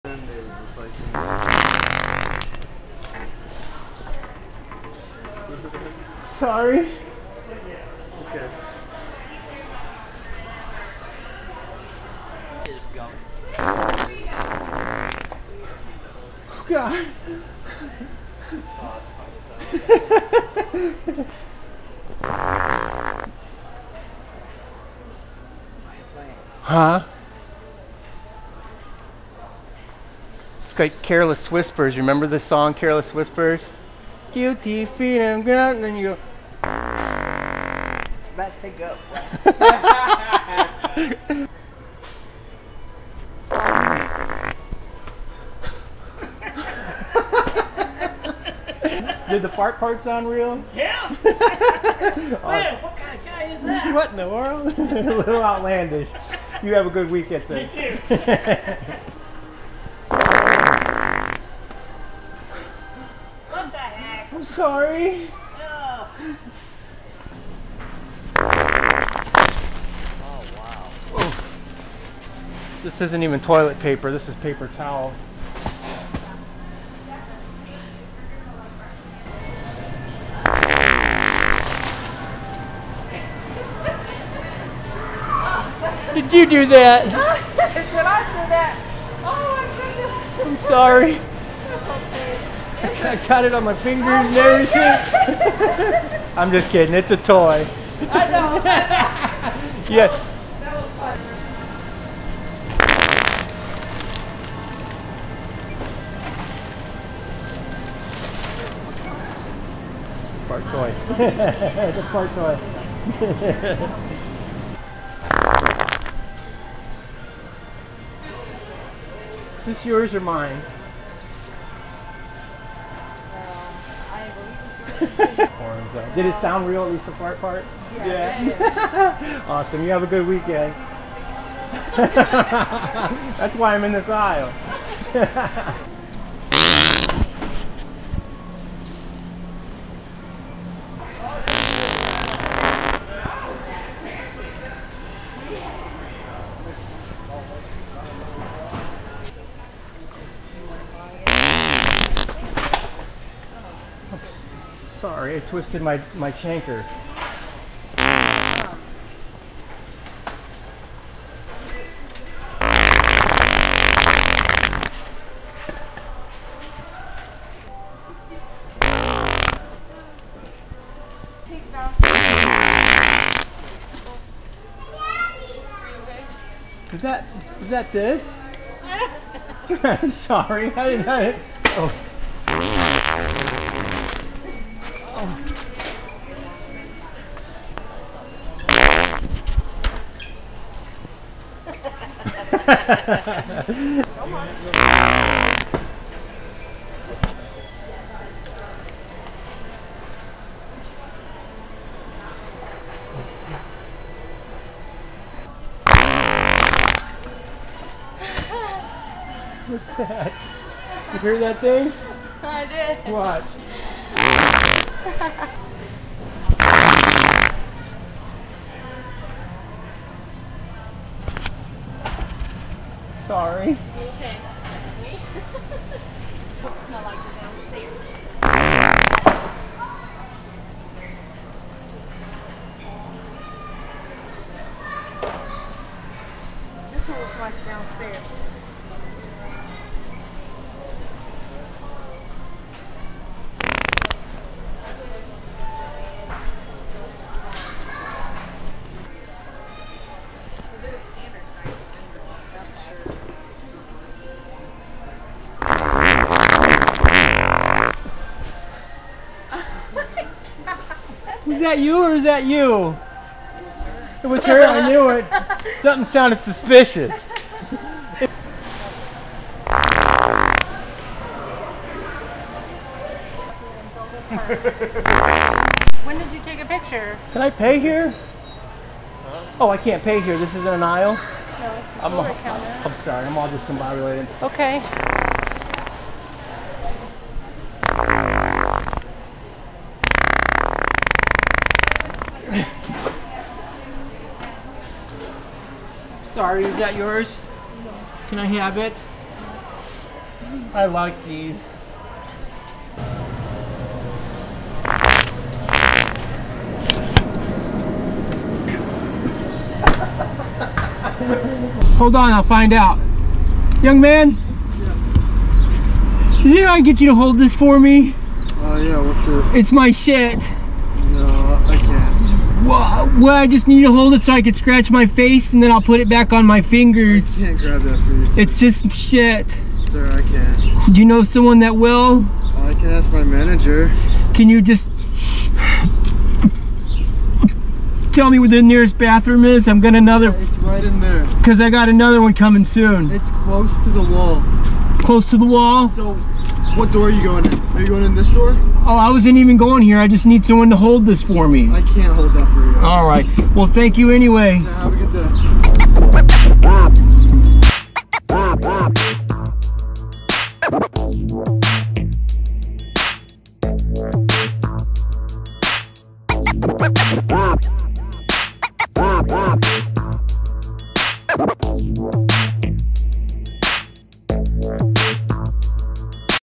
WET FART PRANK - Laugh Along Farting with Poop Fingers!!!
I normally play the crazy guy role with extra antics during my fart prank videos, but this time I decided to just do basic farts and laugh along with my "prank victims" afterwards. I also threw in a few scenes where it looks like I pooped into my hand and have poop on my fingers. I do every kind of fart and poop related prank video you can think of. These farts sound wet because I'm using "The Sharter" fart toy.